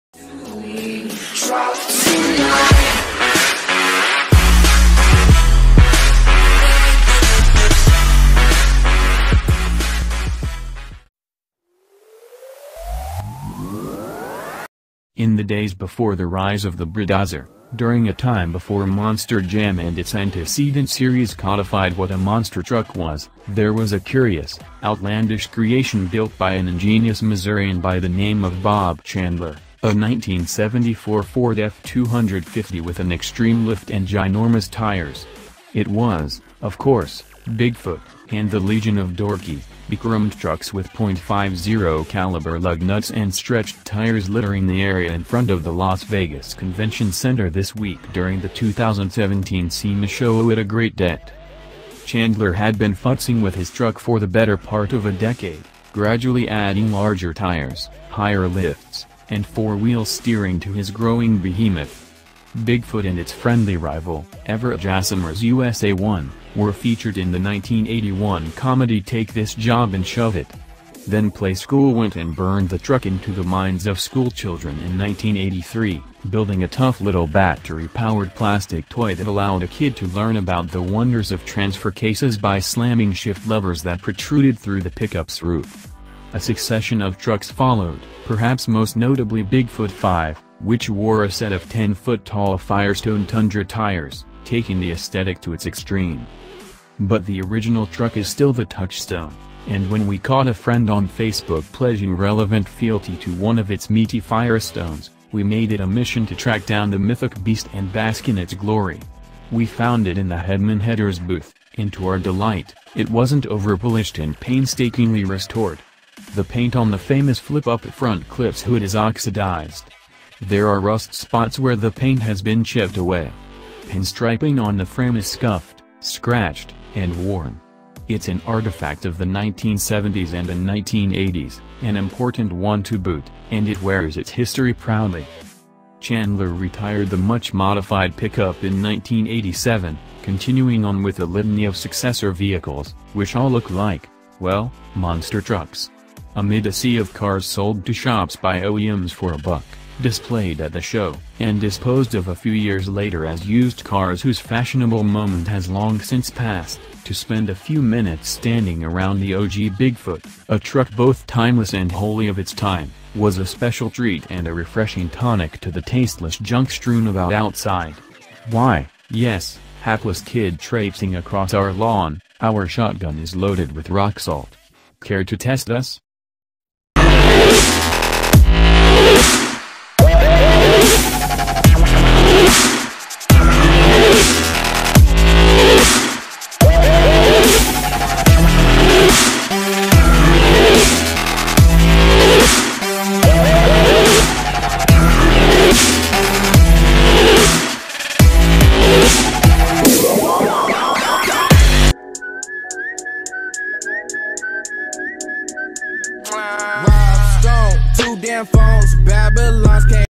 WATCH!!! NEWS FORD BIG FOOT FOR SEMA 2017
WATCH!!! NEWS FORD 250 BIG FOOT  FOR SEMA 2017. In the days before the rise of the brodozer, during a time before Monster Jam and its antecedent series codified what a monster truck was, there was a curious, outlandish creation built by an ingenious Missourian by the name of Bob Chandler: a 1974 Ford F-250 with an extreme lift and ginormous tires. It was, of course, Bigfoot, and the legion of dorky, bechromed trucks with .50-caliber lug nuts and stretched tires littering the area in front of the Las Vegas Convention Center this week during the 2017 SEMA show owe it a great debt.